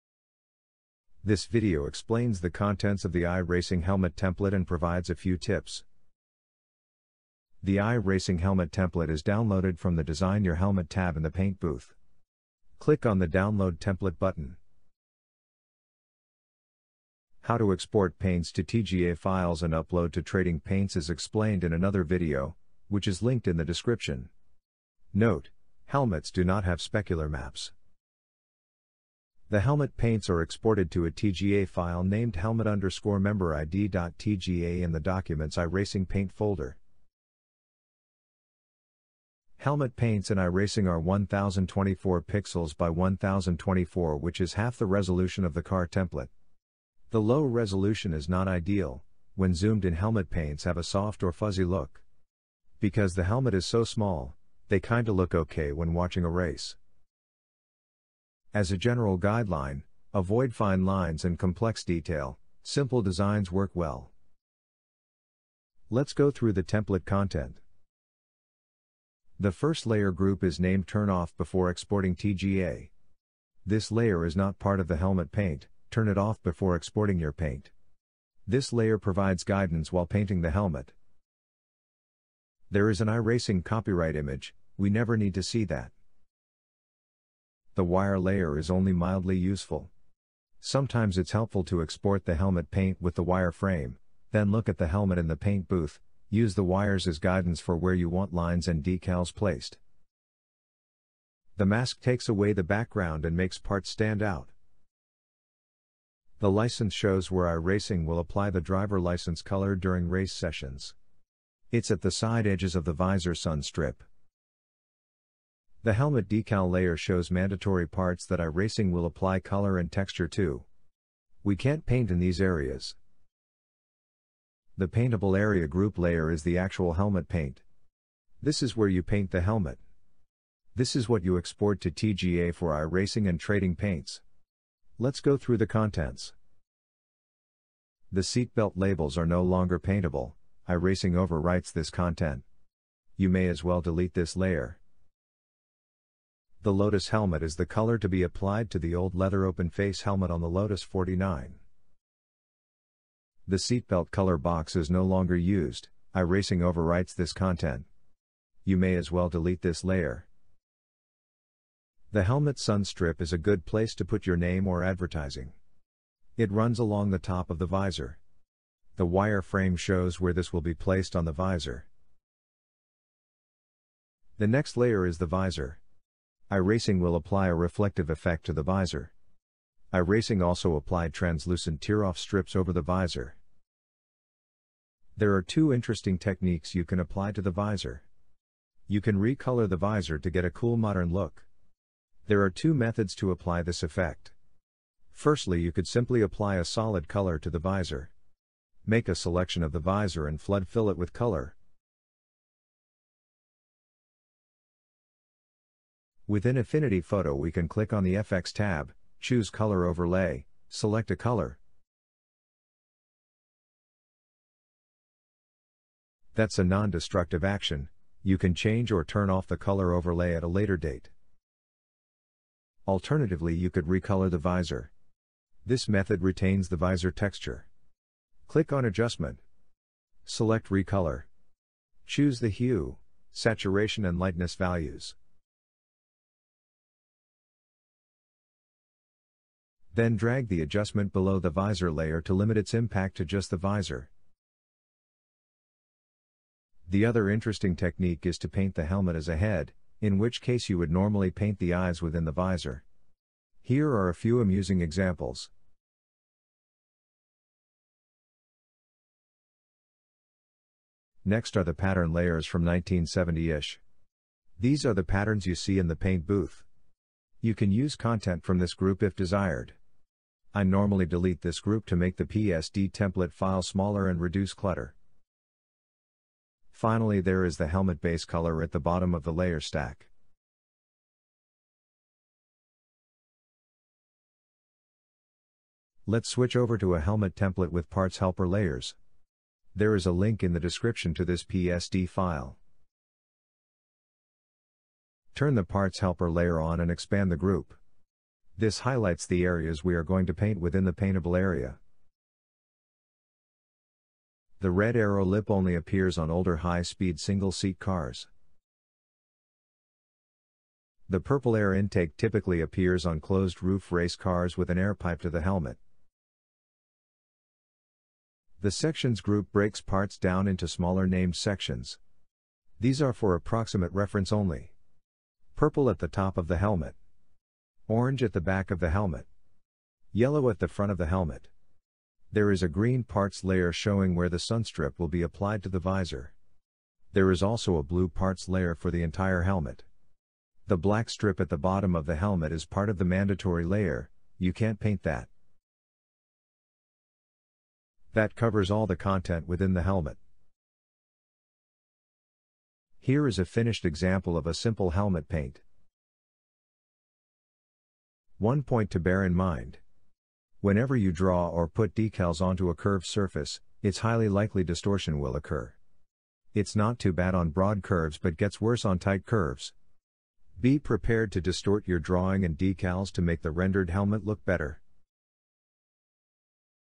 iRacing Paints - Helmet Template (Affinity Photo)
This tutorial explains the contents of the iRacing helmet template.

Where to download iRacing templates and how to export paint into iRacing and TradingPaints is explained here:

Link to PSD file with parts helper.